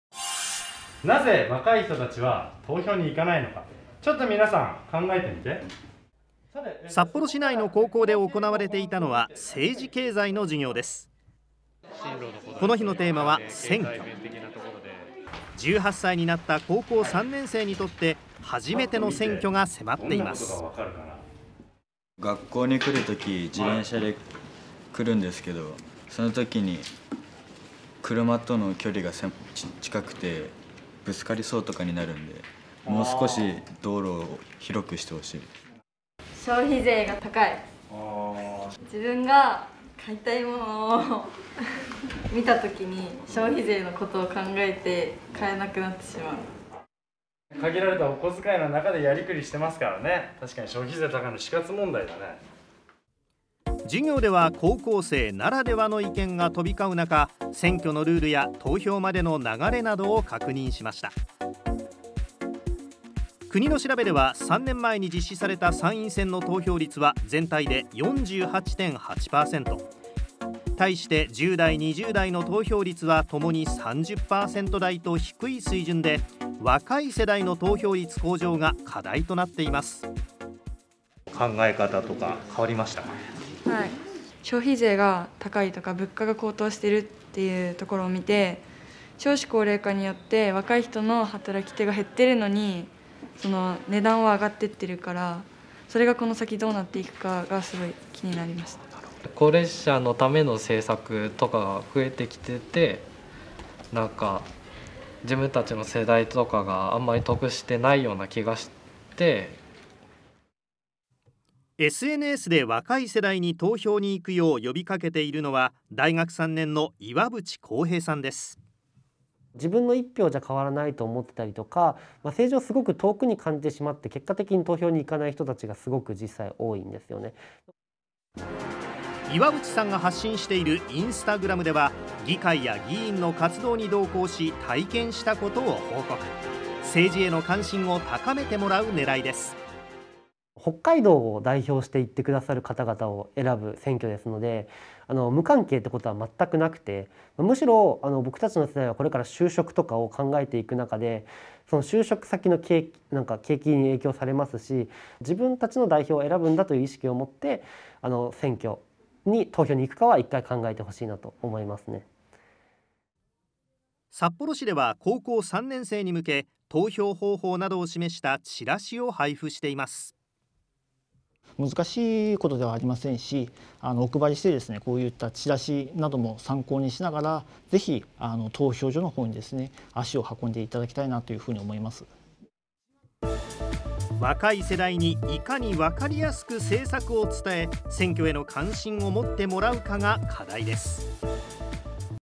【若者の本音】「消費税が高い」「得をしていない」参院選に向け１０代は何を思う？
来月１０日に投開票を迎える参院選。
１８歳選挙権の導入から６年が経ちますが、課題となっているのが若い世代の投票率をどう上げるかです。
若者は選挙をどのようにとらえているのか、学生の本音を取材しました。